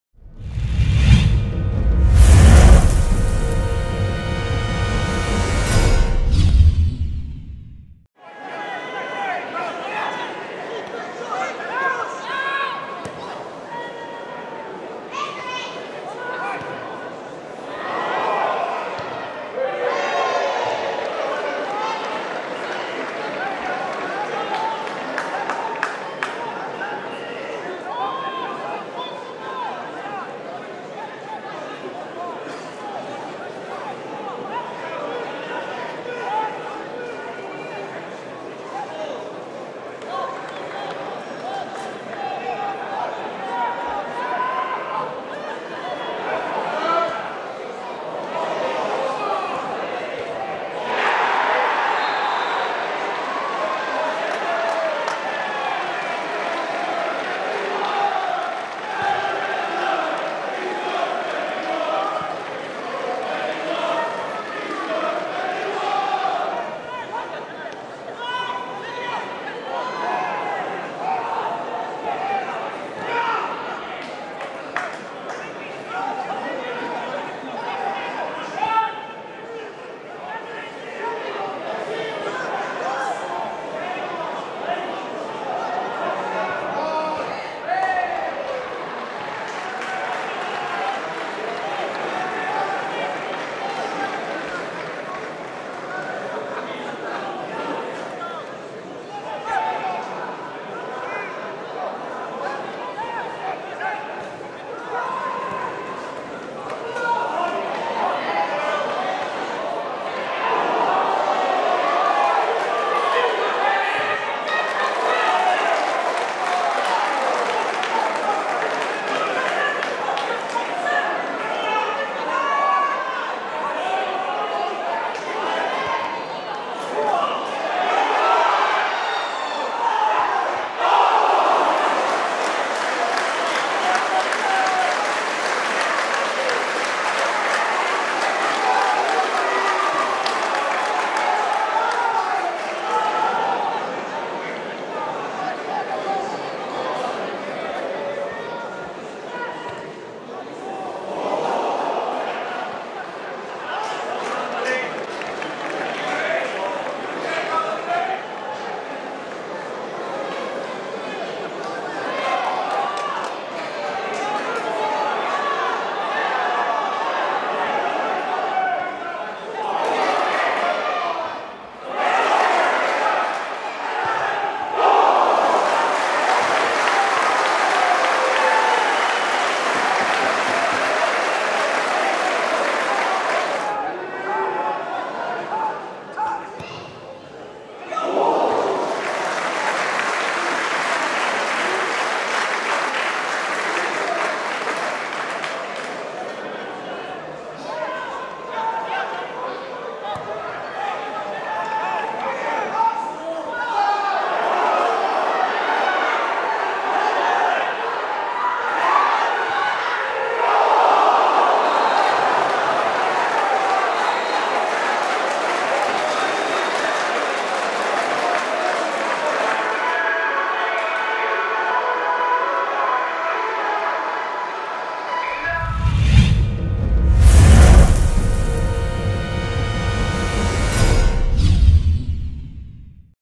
Dundee FC v Inverness CT, League Cup highlights, 24/09/2013
Highlights from the mid week clash in the Scottish Communities League Cup between Dundee FC and Inverness Caledonian Thistle FC at Dens Park. Inverness progress to the next round after winning by 1 goal to nil on the night